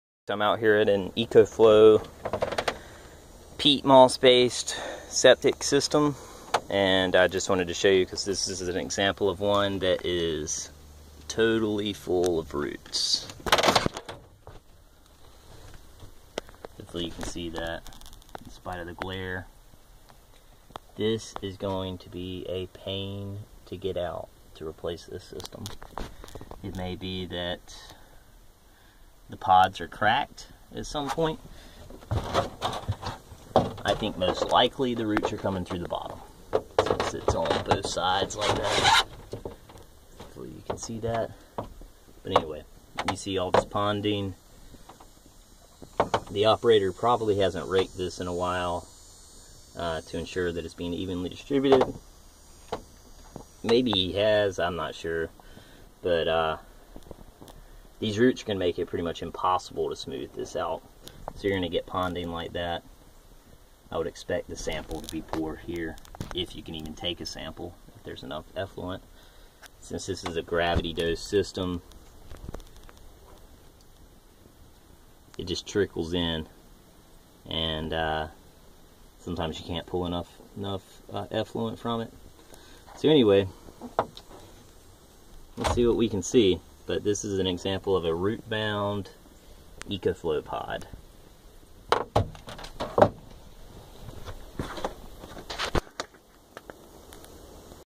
Ecoflo peat septic system root intrusion
An ecoflo that has been invade by roots. Is the pod cracked? Are the roots coming I'm through the bottom? Either way the peat looks pretty bad and will be more difficult to dig out and replace.